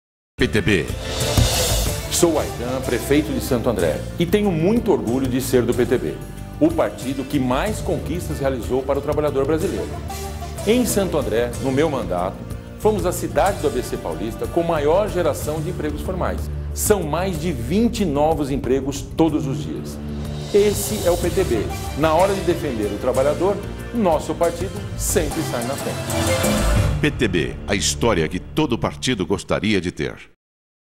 Aidan Ravin (Prefeito de Santo André)
Vídeo Institucional do PTB São Paulo (30s) - Lei 9096/95
Produção: DVM Comunicação (Durval Monteiro)
Imagens: Clipe Produções (Ronaldo Graf)